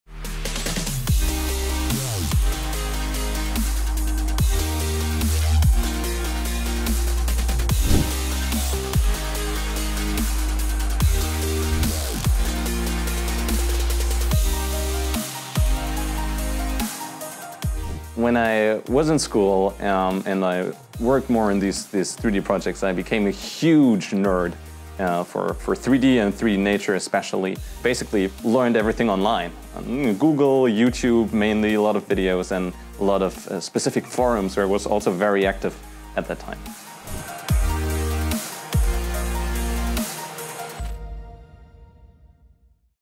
Startup: MY JOURNEY FROM STUDENT TO CEO I Graswald - Start-up Stories by HTGF [Part 2]
👨‍🎓 From student to CEO 👨‍💻 – PART 2!

😍 IT'S TIME FOR A BRAND NEW EPISODE StartupStories! Today, Julius, CEO of Graswald, talks about a topic that aspiring founders in particular are dealing with: Where do I get my information to solve all these challenges?! 🤔

💡 The answer is simple, but brilliant: Become a founding-nerd!

👋 Spending lots of time on startup challenges is important - and nowadays you're just a few clicks away from the biggest library in the world.

🚨 ALERT - next week we're looking forward to hearing more from Julius & Graswald - so, follow HTGF.VC and never miss another episode of our startup stories! 🔔